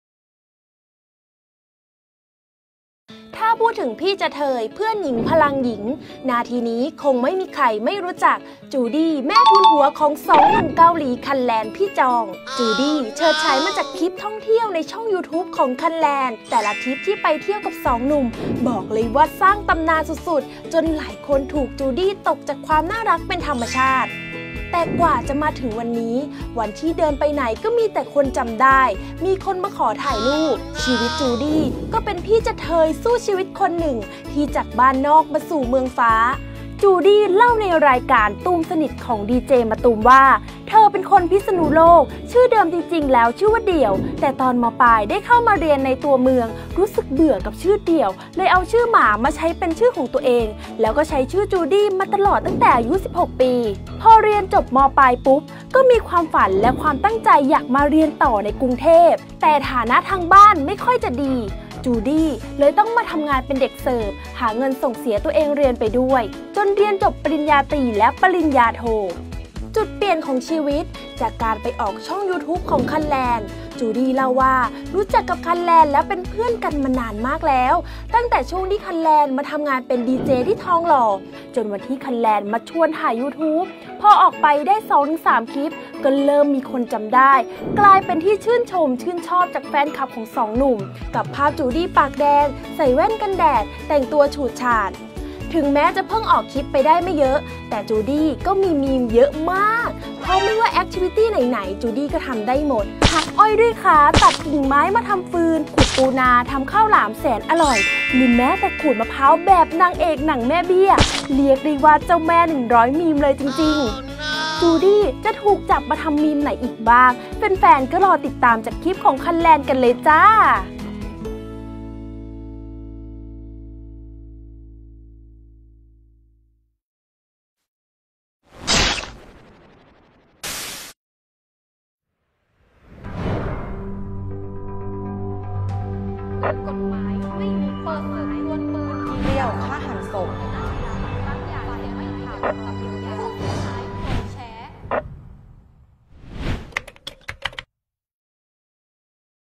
จูดี้ เพื่อนหญิงหนึ่งเดียว ท่ามกลาง 2 หนุ่มเกาหลี คัลแลน - พี่จอง | อีจัน EJAN
นาทีนี้คงไม่มีใครไม่รู้จัก จูดี้ ตัวมัมหนึ่งเดียวท่ามกลาง 2 หนุ่มเกาหลี คัลแลน - พี่จอง